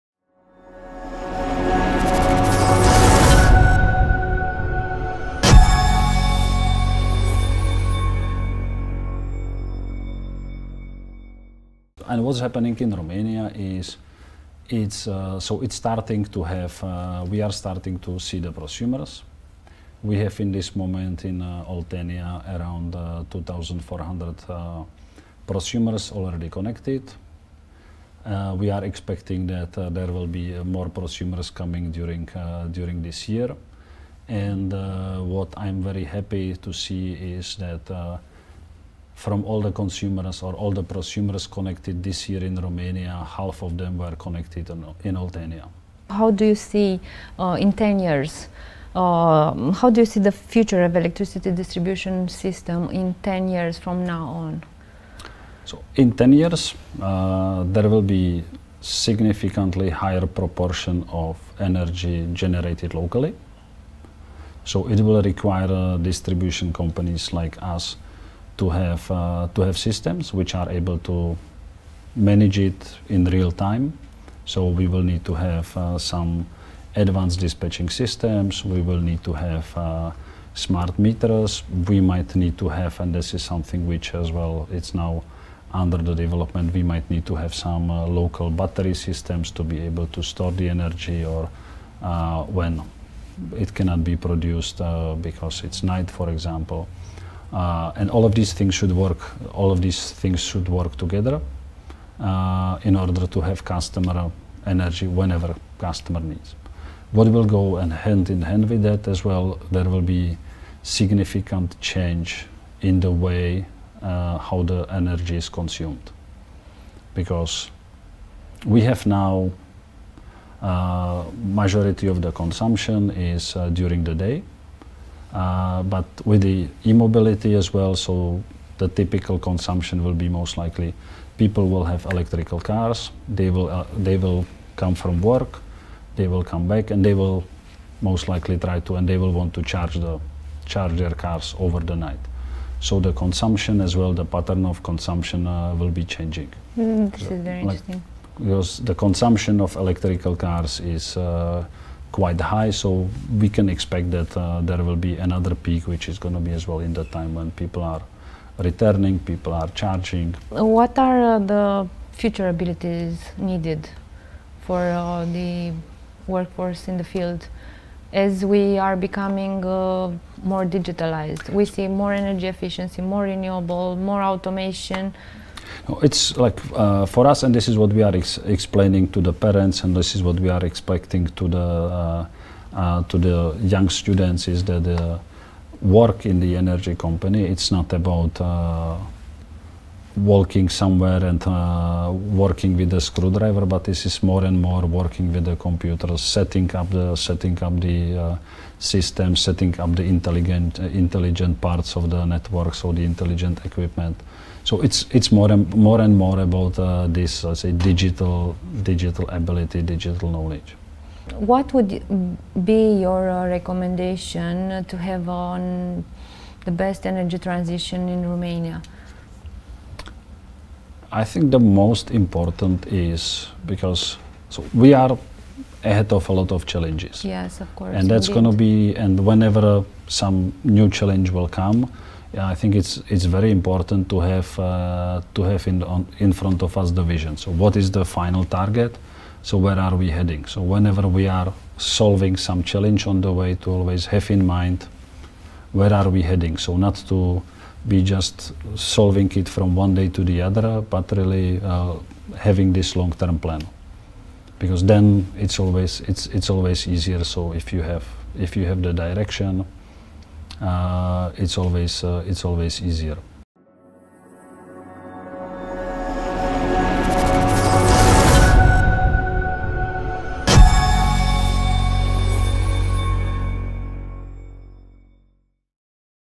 Energy Trends – Ondrej Safar, Country Manager CEZ România (2)
Transformarea digitală prin care trece sectorul energetic schimbă și abilitățile necesare forței de muncă în domeniu. „Munca într-o companie energetică nu este să lucrezi undeva cu șurubelnița, ci este din ce în ce mai mult să lucrezi cu computerele, să configurezi părțile inteligente ale rețelelor. Așadar este tot mai mult și mai mult despre competențe digitale”, explică Ondrej Safar, Country Manager CEZ România.